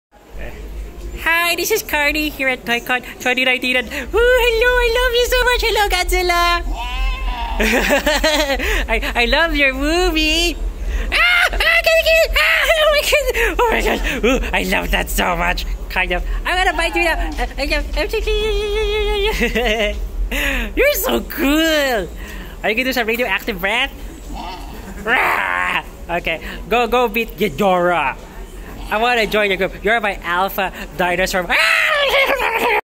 Death of Carny! Eaten by Godzilla
Carny gets eaten by Godzilla!  Oh no!

(actually, Carny was suppose to be retired after this video but he stuck around anyhow, whew)